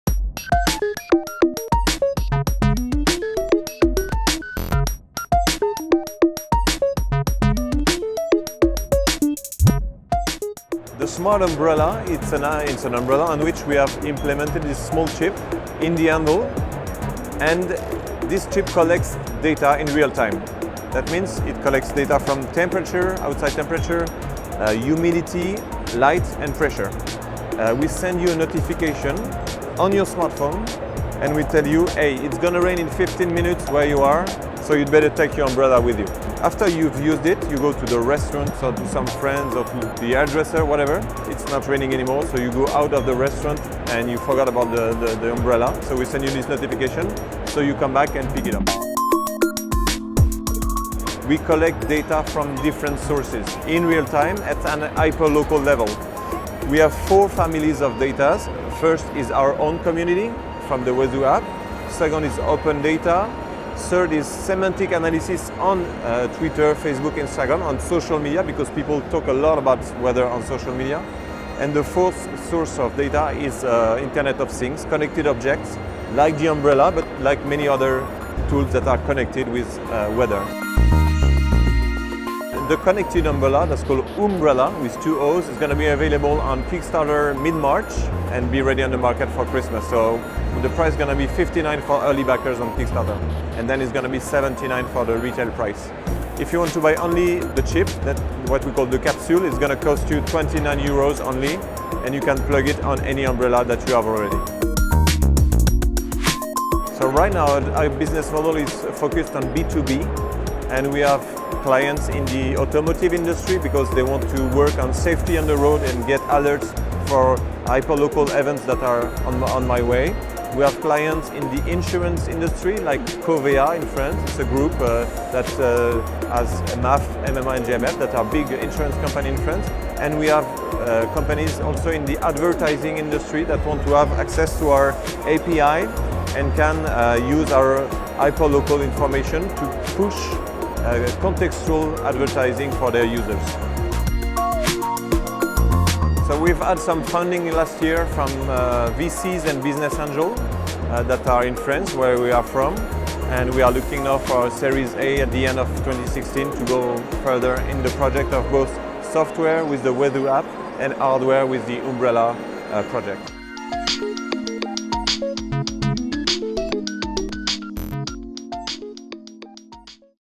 The Umbrella That Tells You When It Will Rain
A connected umbrella that uses real-time weather data claims it will notify you before it rains. The makers of the device demonstrated it at the 4 Years From Now event at Mobile World Congress in Barcelona.